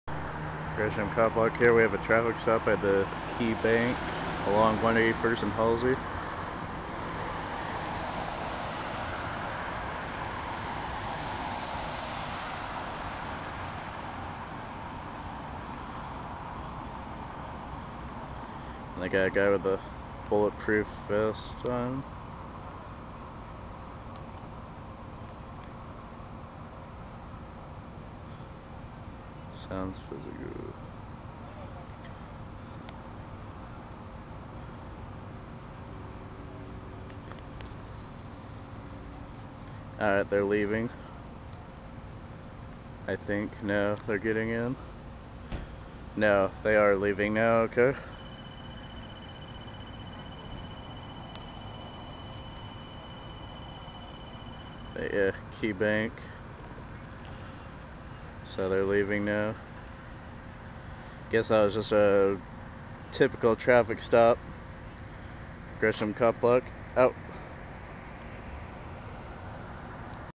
Undercover Gresham PD Traffic Stop on 181st Near Halsey
This was an incident right after the situation where the Gresham PD stormed inside and guarded Shari's Restaurant with Rifles and on the hunt for a white male walking around with a gun. This was probably the shortest traffic stop ever.